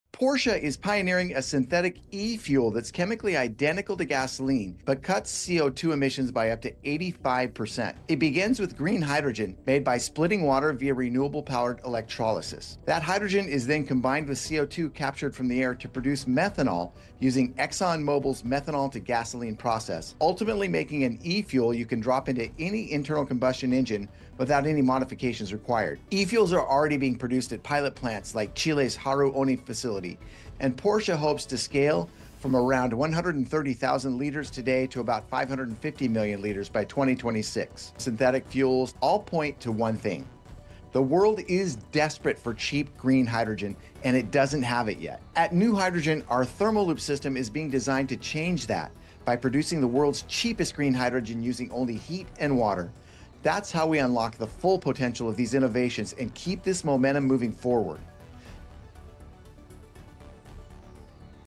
This Synthetic Fuel Looks Like Gas… But Isn’t
Porsche’s eFuel looks like gas—but with up to 85% fewer emissions.

 It starts with green hydrogen and captured CO₂, then turns into a drop-in fuel that works with any gas-powered car. Pilot plants are already online, but scaling this solution means one thing: we need cheap green hydrogen—and fast.

That’s where NewHydrogen’s ThermoLoop comes in.